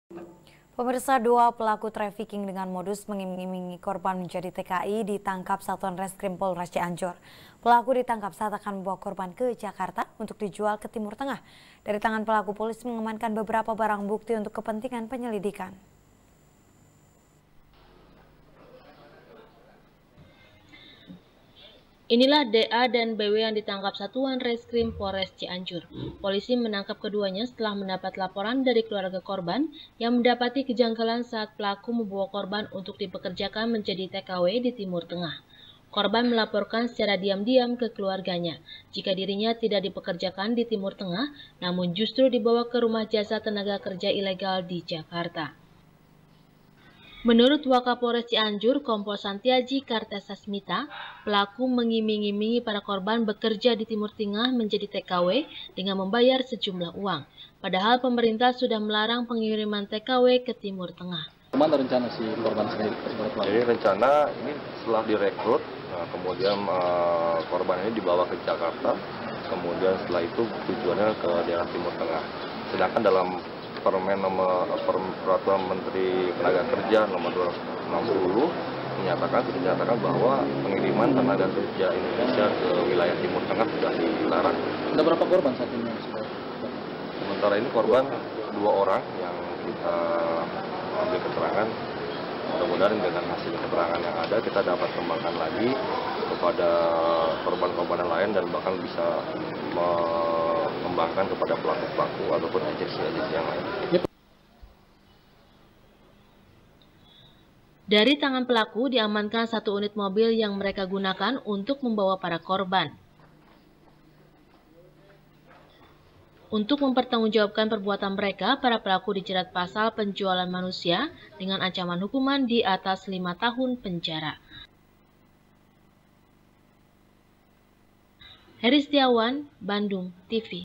2 Pelaku Trafficking TKW Ditangkap | SBR | BANDUNG TV
SBR | BANDUNG TV | NIKEN PRATIWI | Dua pelaku trafficking dengan modus mengiming-imingi korban menjadi tki, ditangkap satuan reskrim polres cianjur. pelaku ditangkap saat akan membawa korban ke jakarta, untuk dijual ke timur tengah. dari tangan pelaku, polisi mengamankan beberapa barang bukti untuk kepentingan penyelidikan.